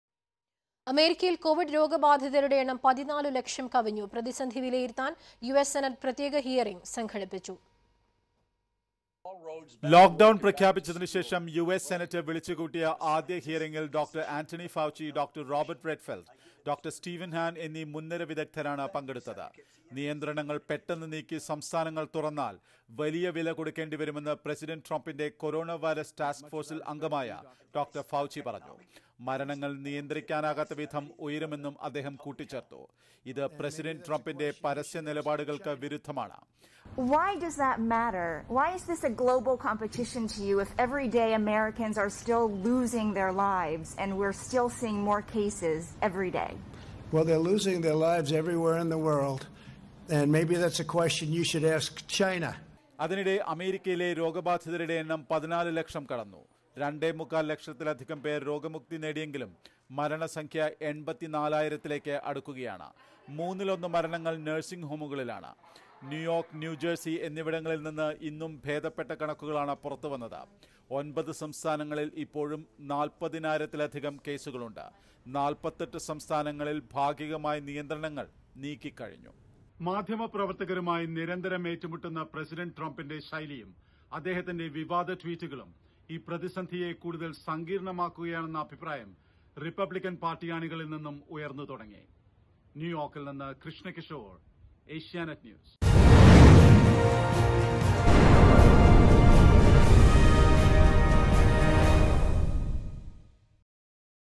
അമേരിക്കയിൽ കൊവിഡ് ബാധിതരുടെ എണ്ണം 14 ലക്ഷം കവിഞ്ഞു America COVID 19
America covid infection rate 14 lakhs